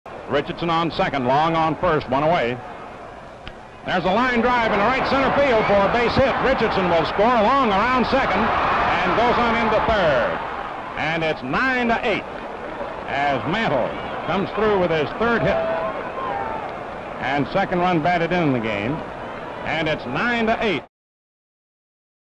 1960 WS Gm7: Mantle cranks an RBI single in the 9th of Game 7
10/13/60: Mickey Mantle hits an RBI single in the top of the 9th to score Bobby Richardson, cutting the Yankees' deficit to one in Game 7 of the World Series